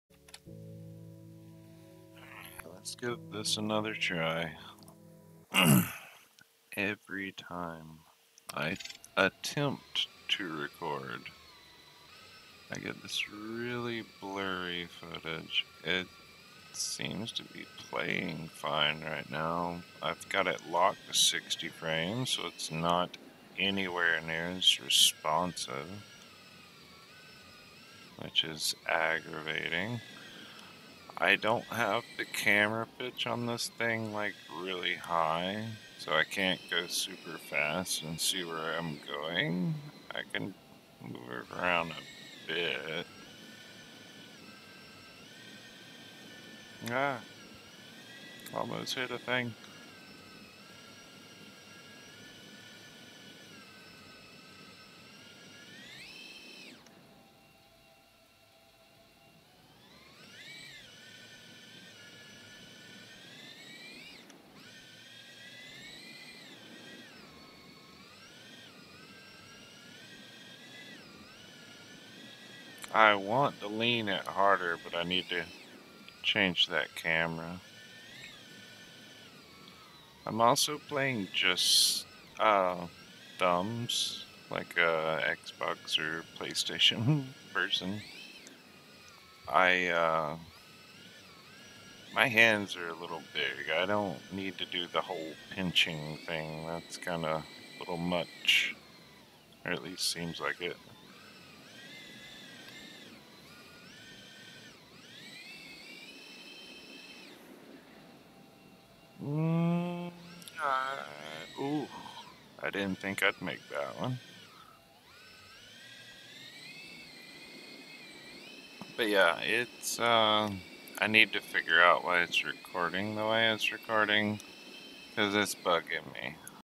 Stuttering DLSS RTX Recording (3080TI)
Everything else has been recording fine. Just DLSS/RTX titles.. And only after my last GPU driver update 472.12 that was necessary for another game.

I'll sort the issue, just wanted to give an excuse for why I am not uploading my drone sim and other new games content. Same thing happening in Forza Horizon 5 and gameplay is butter smooth.. Sucks but I'll figure it out.